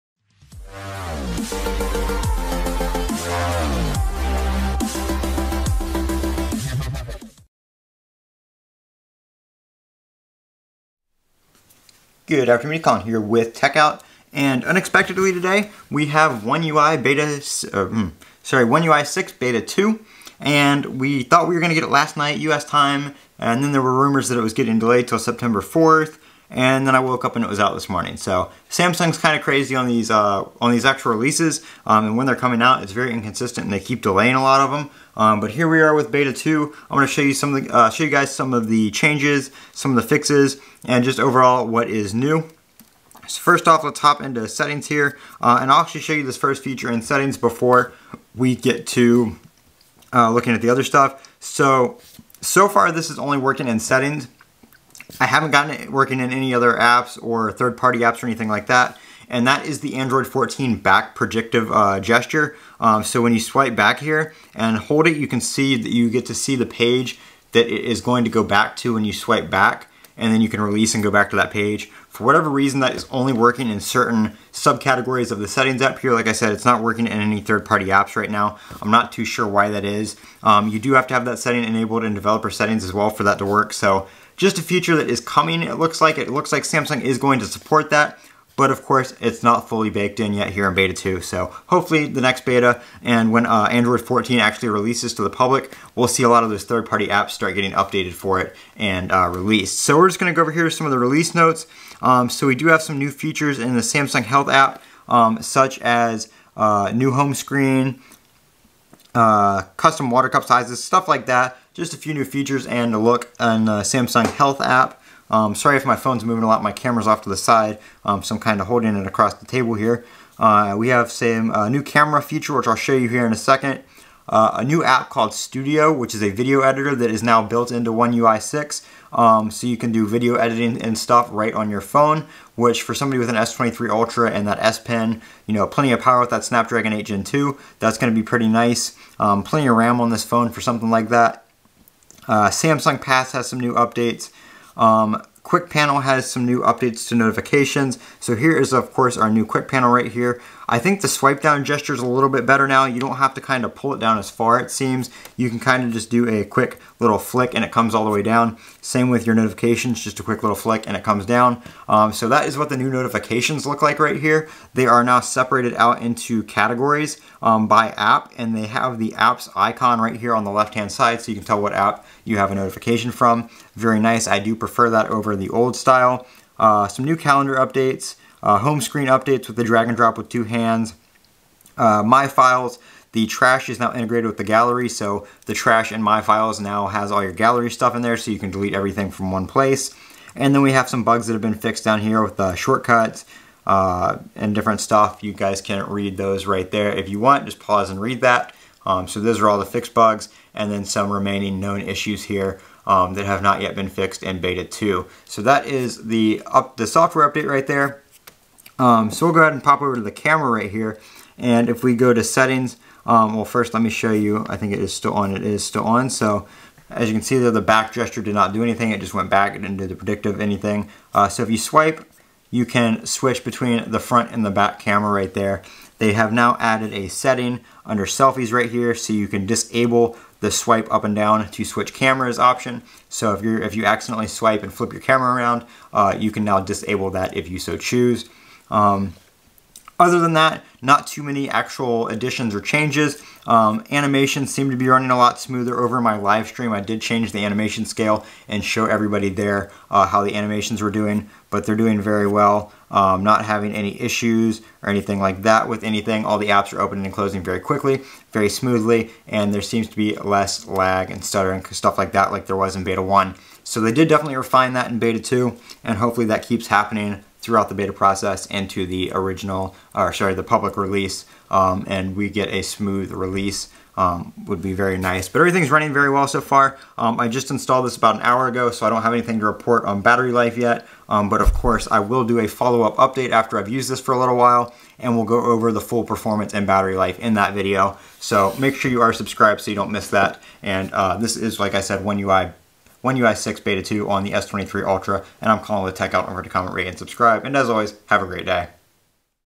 One UI 6 Beta 2 Released With Improved Performance and New Features
Socials:
X/Instagram @TechoutonYT

Prime Student 6 Month Trial

My Gear:
14 Inch M1 MacBook Pro
Mac mini
Monitors